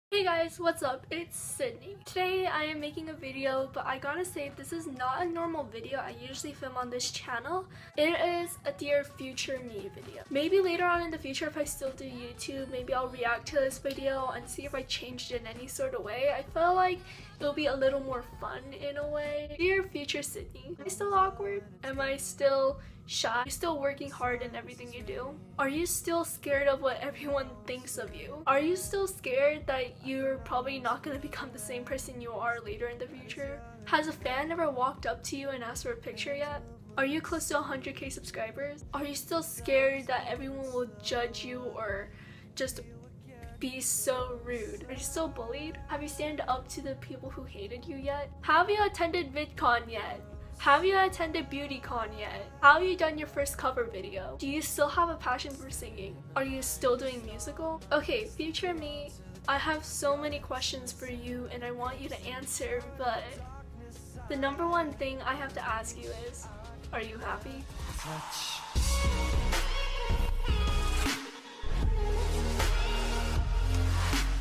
DearFutureMe//Sydney Park
in the end I am asking myself "are you happy" because I am going through something right now that I'm pretty sure none of you would ever imagine me to be going through as a youtuber but anyways I will be going through more of that next week probably but not sure yet. It is a dear past self and I will be going though that more in that video.